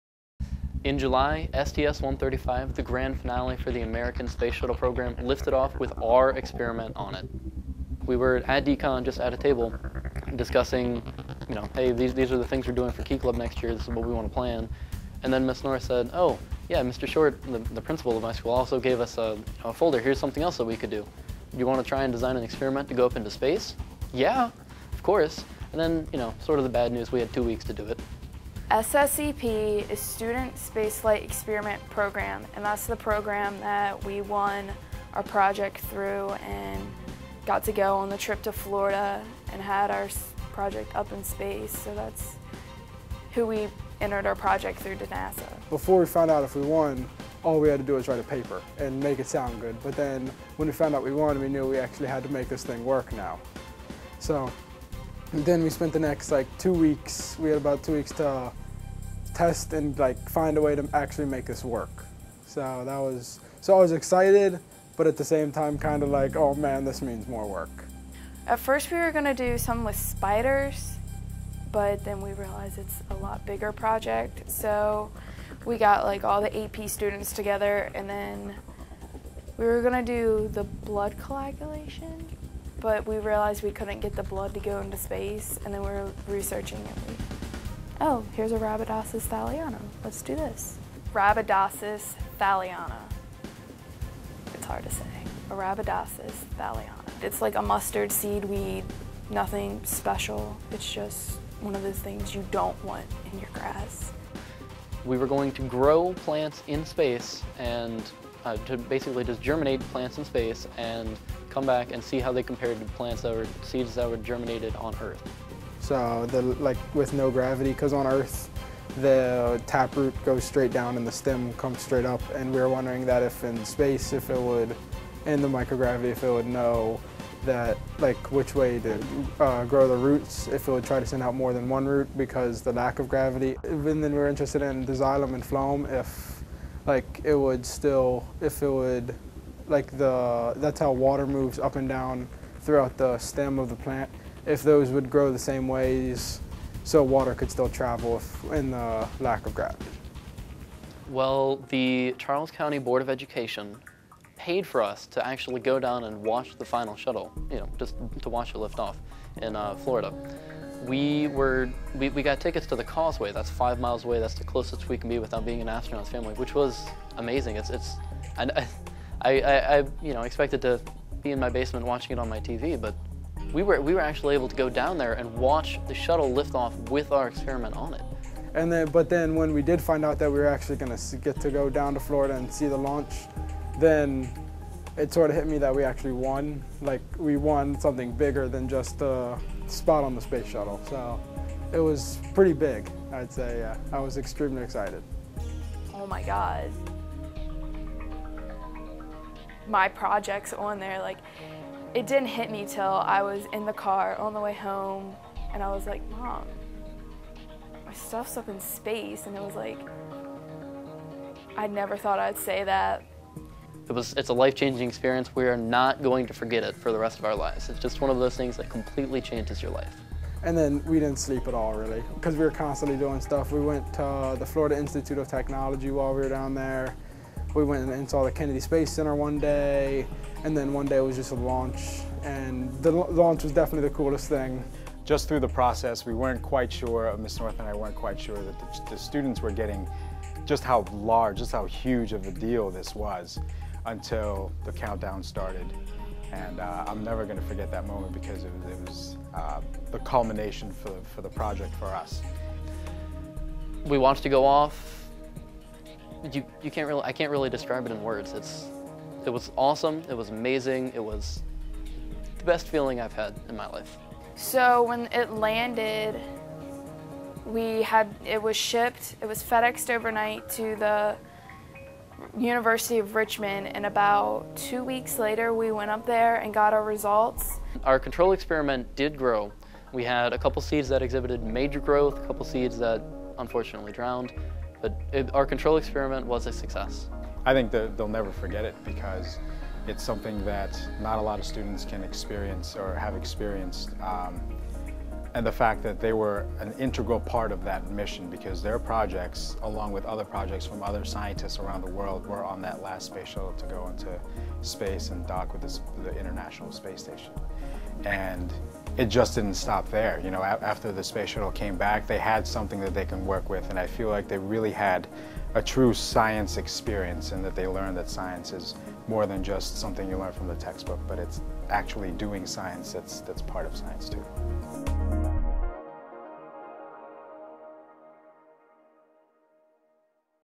Key Club experiment goes into space
Key Clubbers from Henry E. Lackey High School won the SSEP competition to send a science experiment into space on the last mission of the space shuttle Atlantis. Club members along with other students at Lackey High School designed an experiment to compare the germination of arabidopsis thaliana, a small flowering plant in the mustard family, in space to its germination on Earth. After winning the competition, students were able to travel to Florida to see the launch first hand. This is their story.